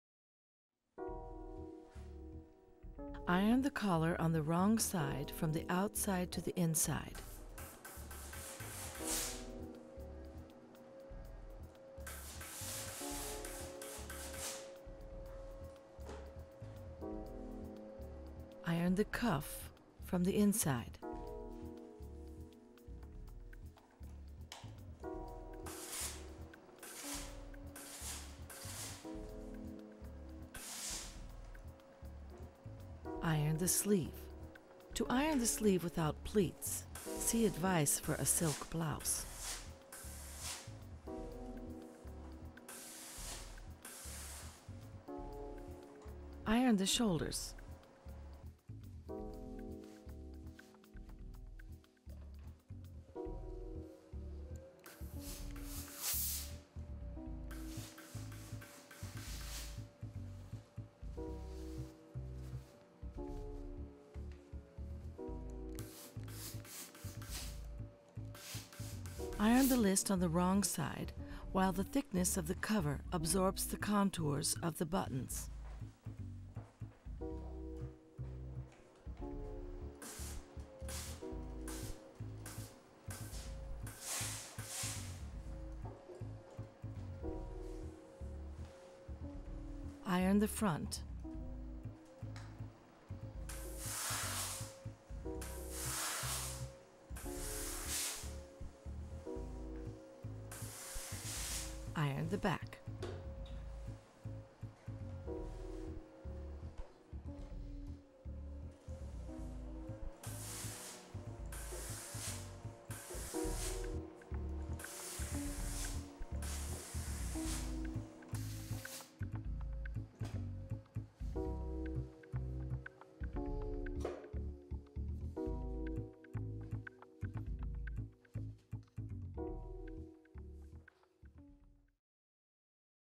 How to iron a shirt in 2 minutes - Laurastar
Discover how to iron a shirt in 2 minutes with Laurastar's professional iron. Your step-by-step guide on how to best use your Laurastar product. for more information.